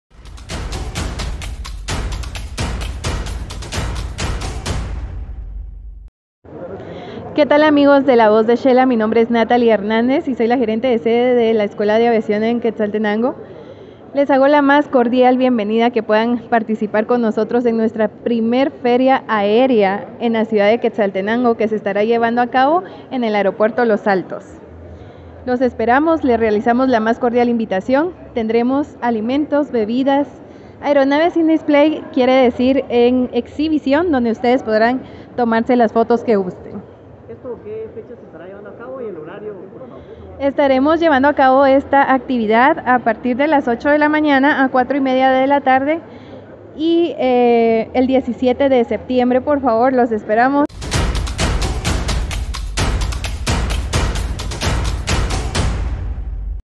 PODRÁS TOMARTE FOTOS CON AERONAVES 🛩️
| Invitan a asistir a la primera Feria Aérea en Xela este 17 de septiembre en el aeropuerto Los Altos en el marco de Xelafer 2022. El evento es organizado por la escuela de aviación Aerotraining.👇
📲📹La Voz de Xela / Leonel Rodas